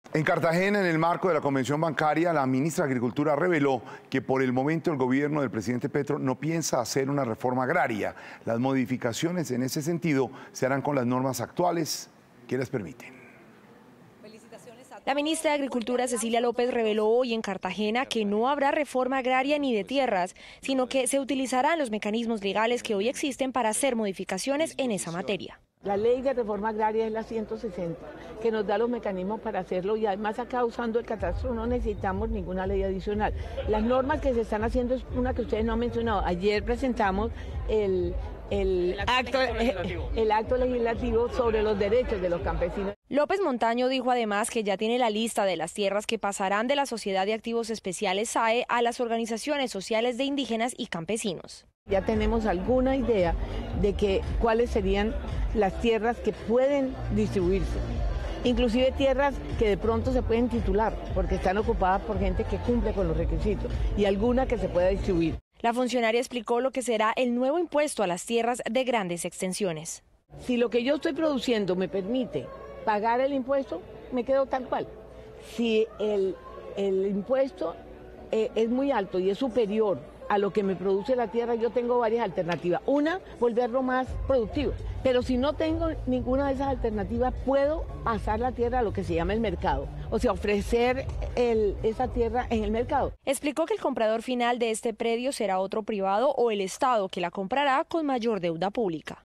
“No necesitamos ninguna ley adicional”: minagricultura reveló que no habrá reforma agraria
En Cartagena, en el marco de la convención bancaria, la ministra de Agricultura de Colombia, Cecilia López, reveló que, por el momento, el gobierno del presidente Gustavo Petro no piensa hacer una reforma agraria.

Las modificaciones en ese sentido se harán con las normas actuales que las permiten.